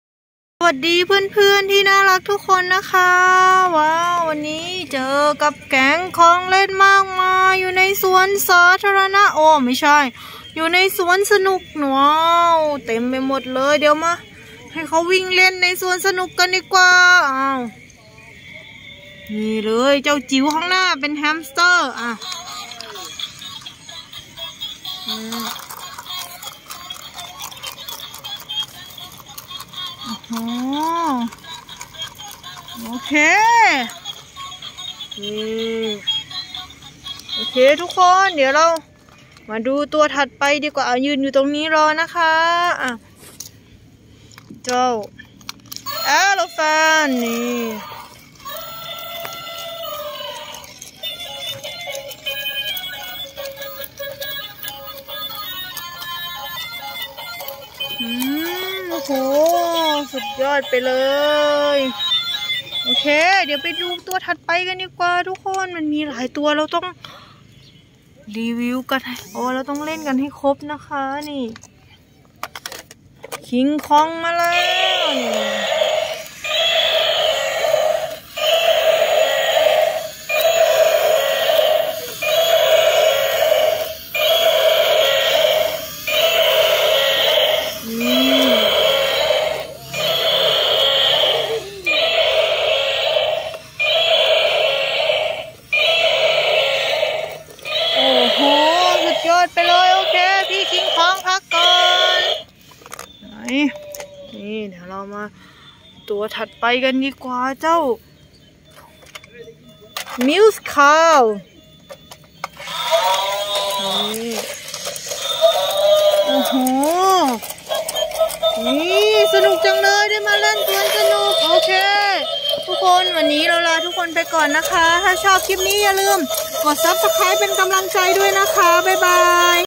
รีวิวของเล่นไดโนเสาร์ และช้างใหญ่ ช้างตัวน้อย ไดโนเสาร์การ์ตูน ลิง ปลานีโม่ คิงคอง
Review of dinosaur toys and big elephants, little elephants, cartoon dinosaurs, monkeys, fish, Nemo, and King Kong.